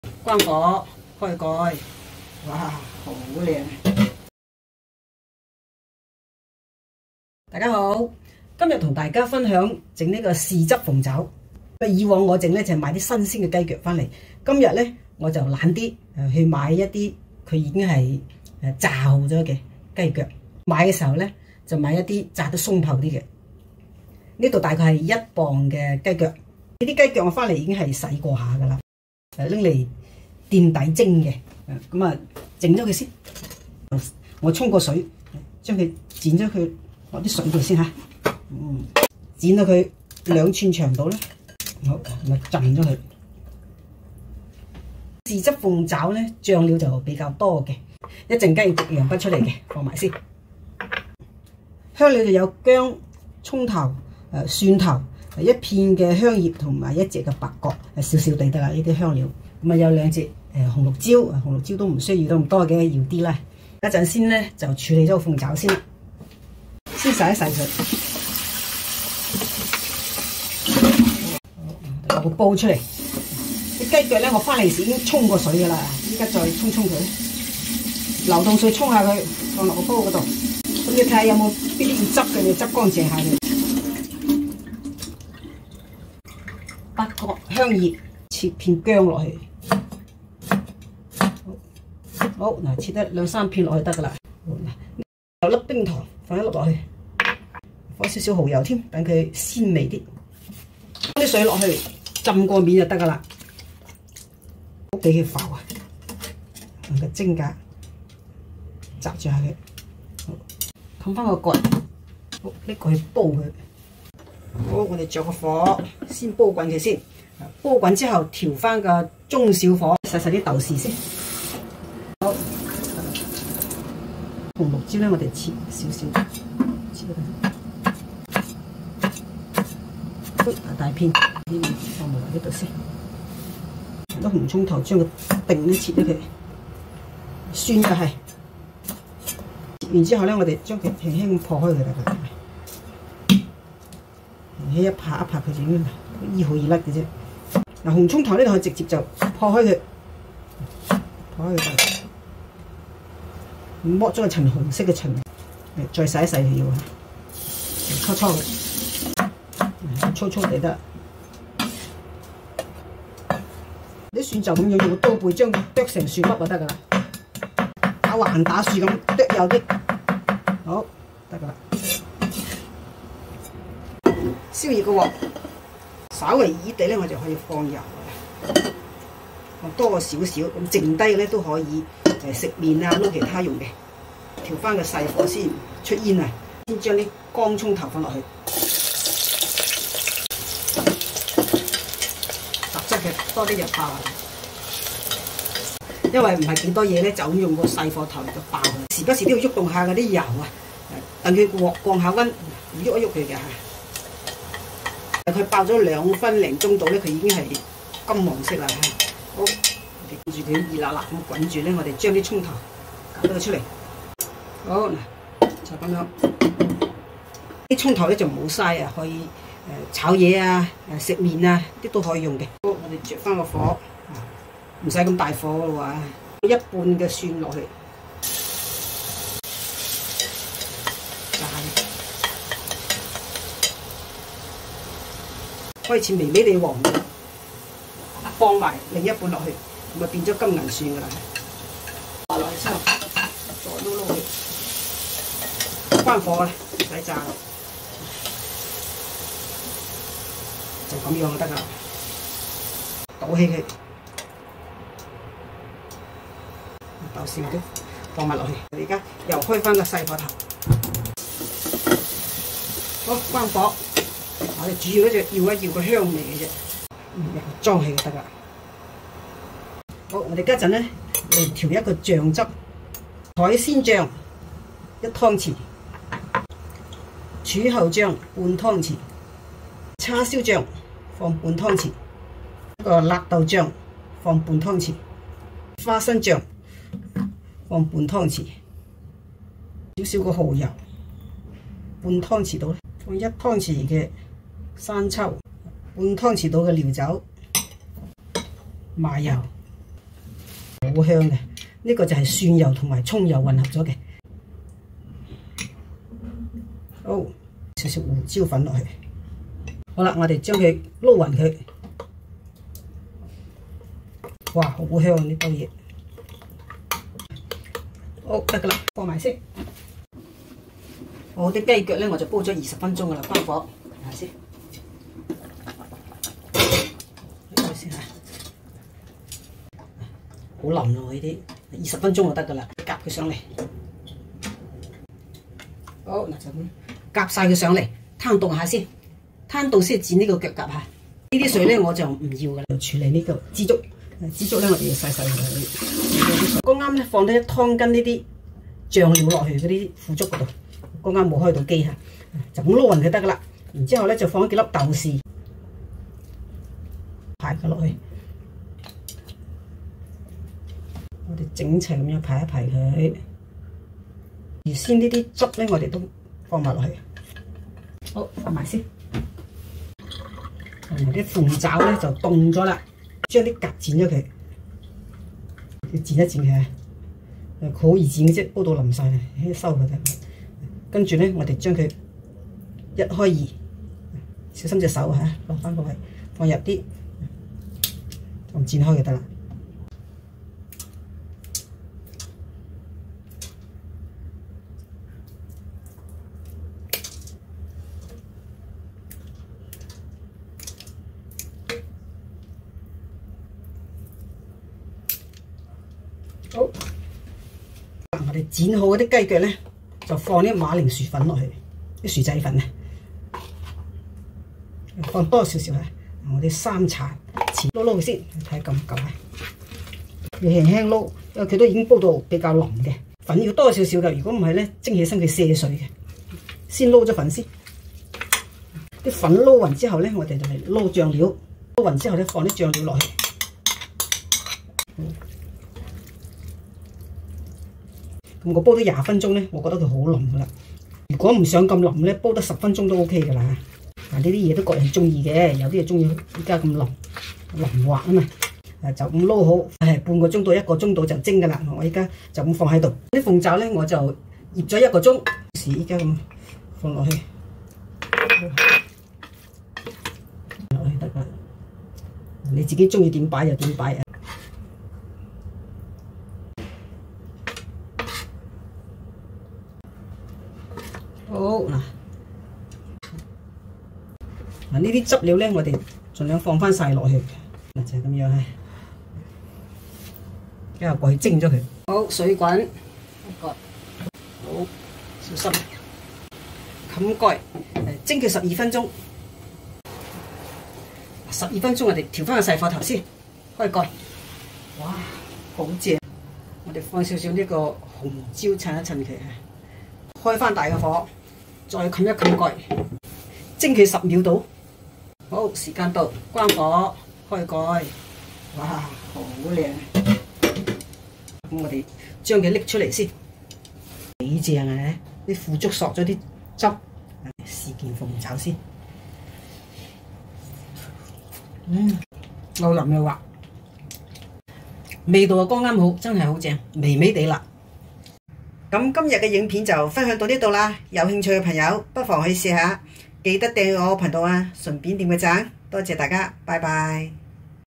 豉汁鳳爪 廣式美食 Chicken Feet in Black Bean Sauce
材料:
雞腳1磅，豆豉，青紅椒各適量。
煲雞腳調味:
姜，香葉，八角，冰糖
混合醬:
海鮮醬，生抽，麻油各1湯匙，侯柱醬，叉燒醬，辣豆醬，花生醬，蠔油，料酒各半湯匙，蔥蒜油2湯匙，胡椒粉半茶匙。